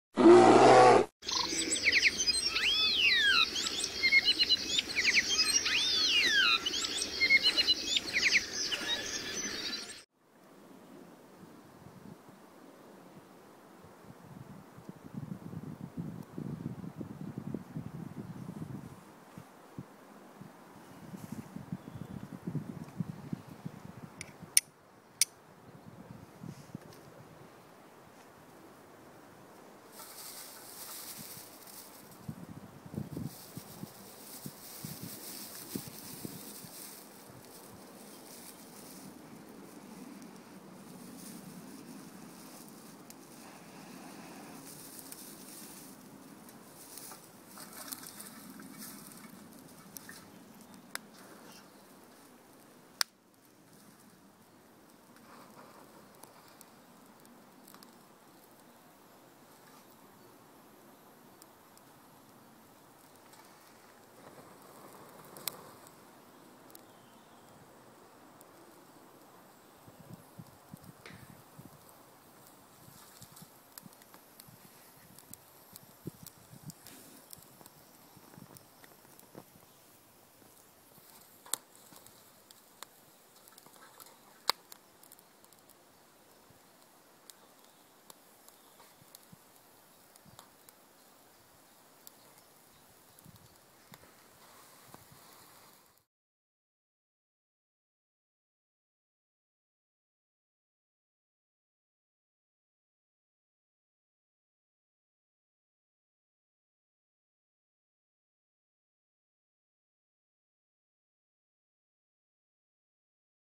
Comment allumer un feu au Briquet médiéval, silex et coton carbonisé/ how to make fire with flint
Allumage d'un feu avec un briquet à silex et cotton carbonisé.